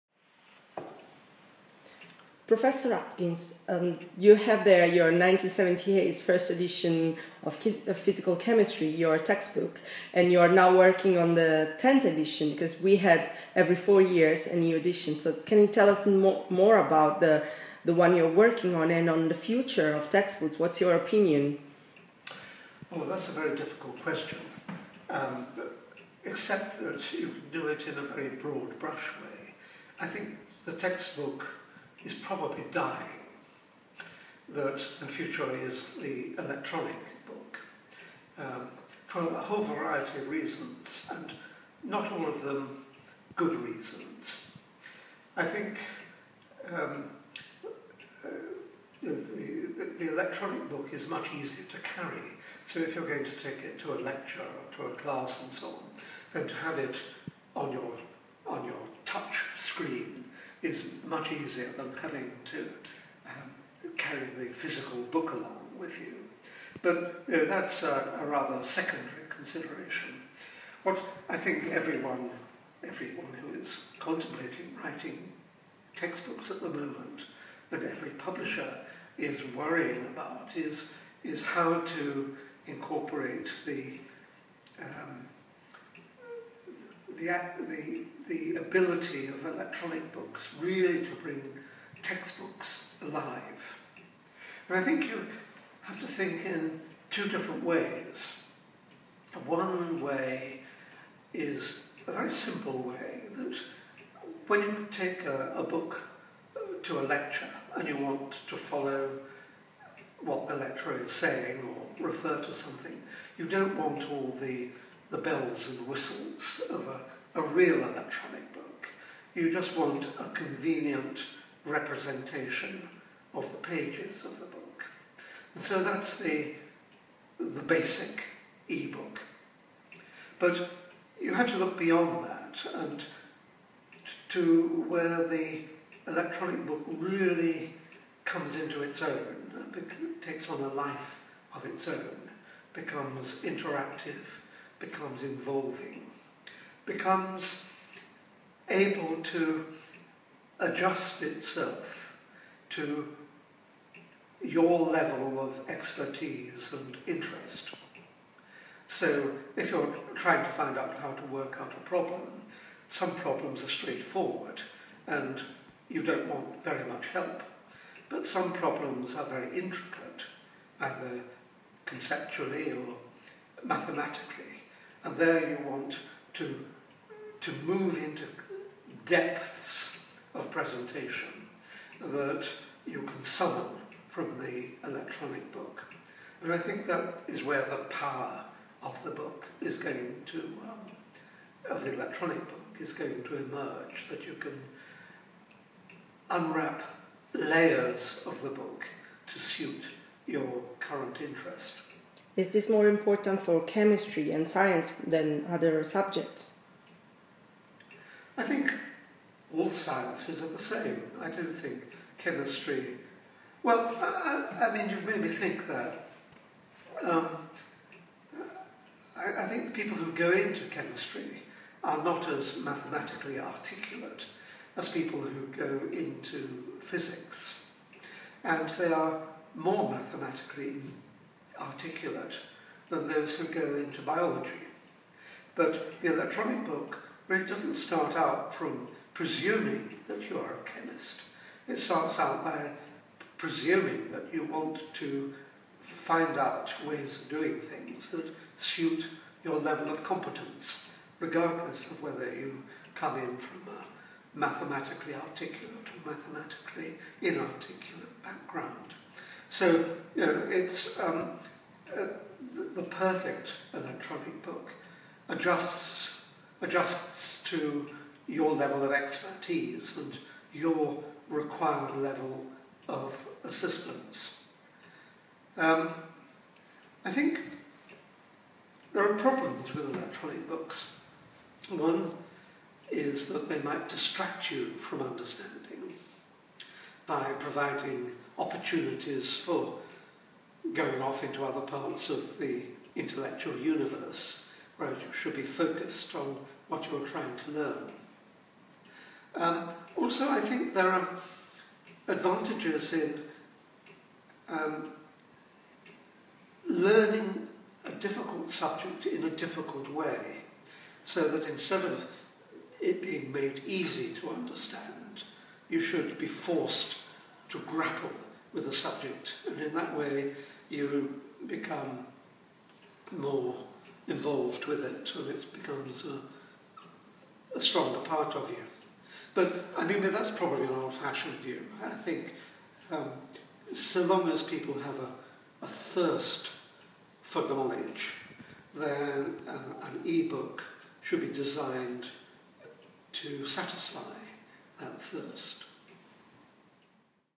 Peter Atkins on the future of textbooks
Professor Atkins, distinguish chemist at University of Oxford and author of "Physical Chemistry" interviewed at the Chemistry Centre on the future of textbooks